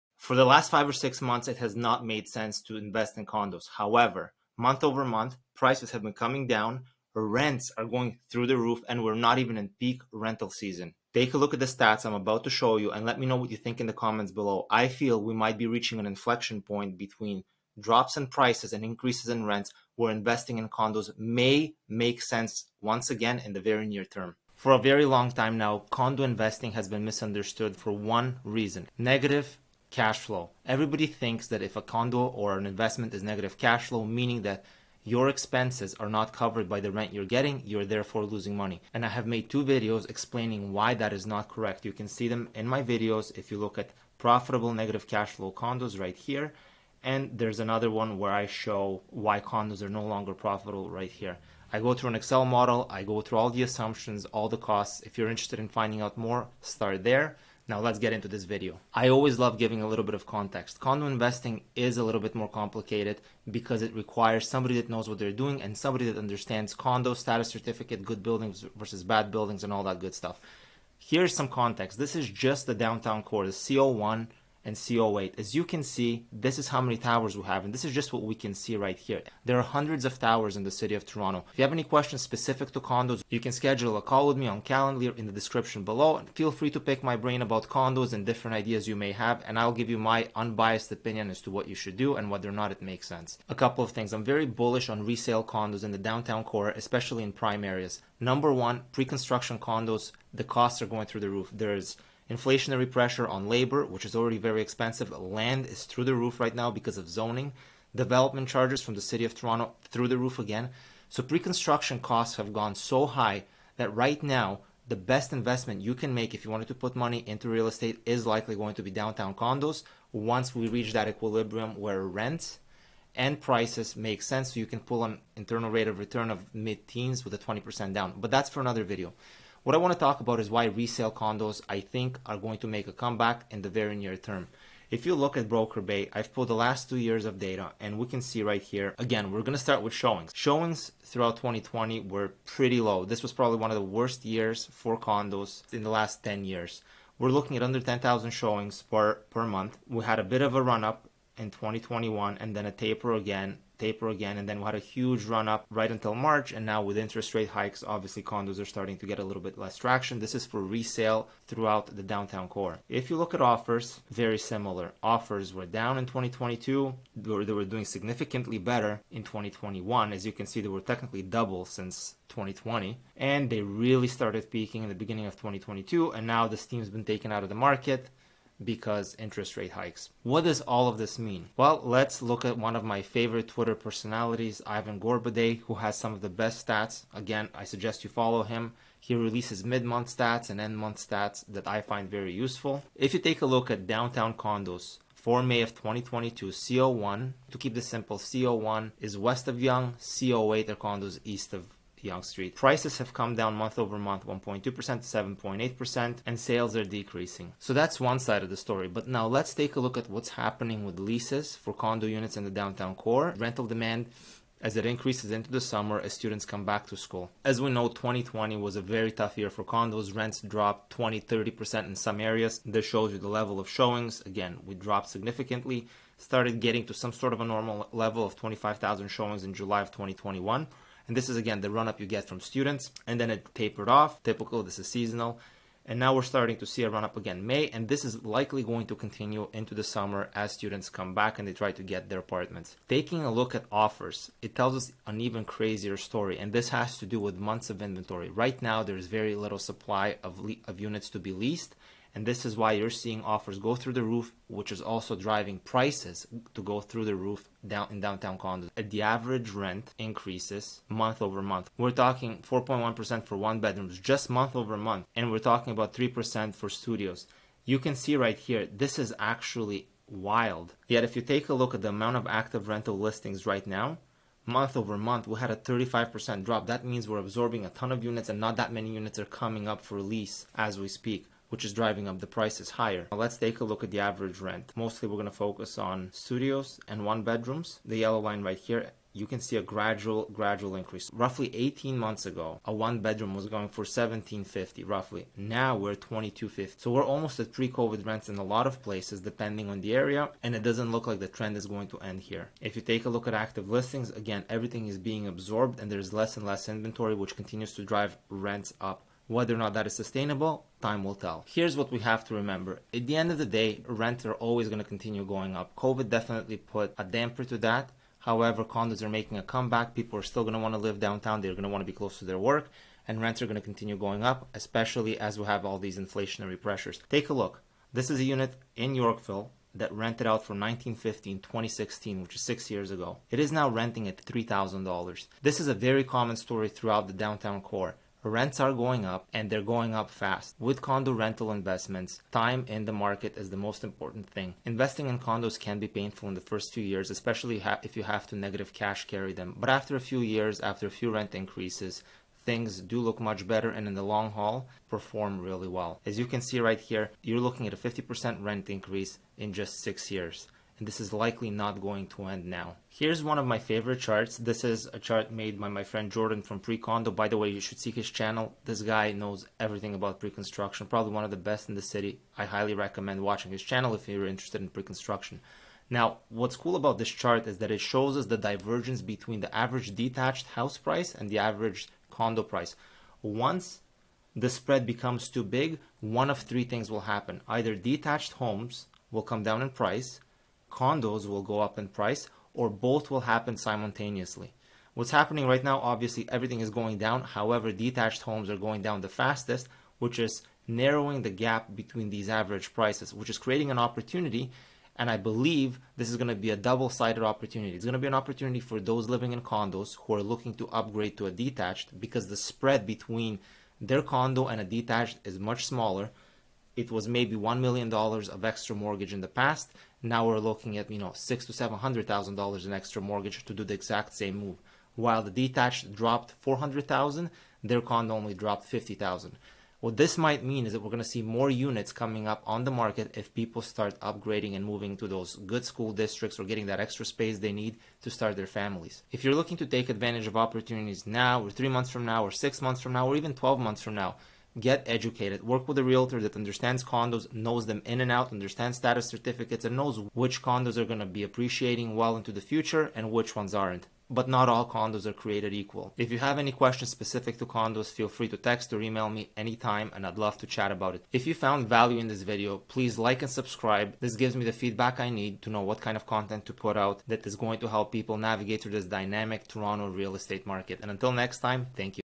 Are Toronto Condo Investors coming back?
In this video we review the current state of Downtown Toronto Condos, the decrease in prices and rapidly increasing rents.

Are there buying opportunities on the horizon for Real Estate investors, that’s what early trends are telling us.

Vassil Staykov, CPA, Realtor® | City Estates